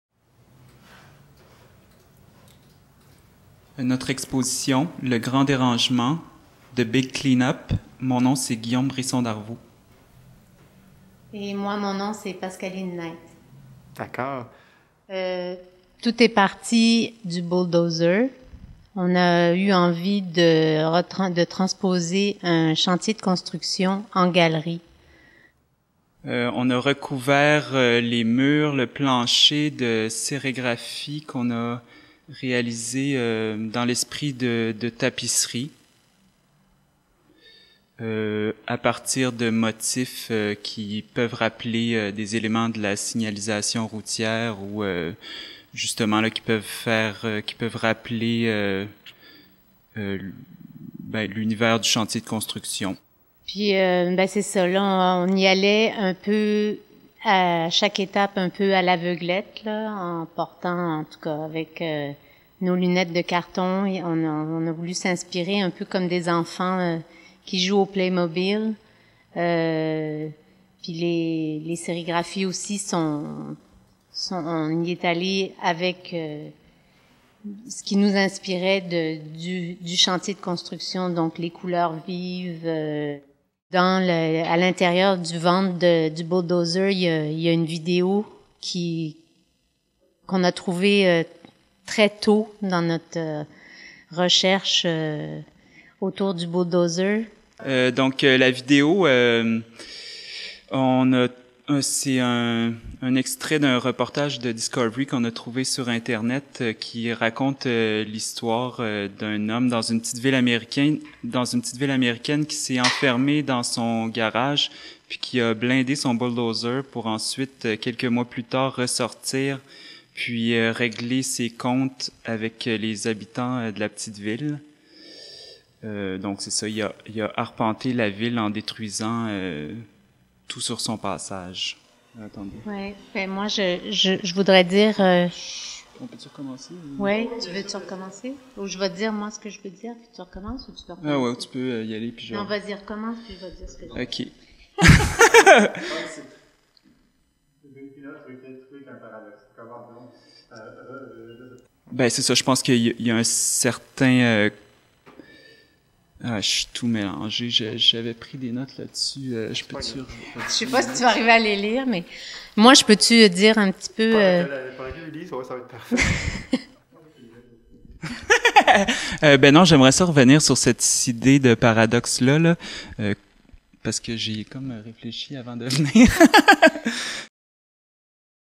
Brisson-Darveau & knight
Entretien avec Guillaume Brisson-Darveau & Pascaline Knight dans le cadre de leur exposition «Le Grand Dérangement/The Big Clean-up» chez ARPRIM, 372 rue Sainte-Catherine O., espace # 426,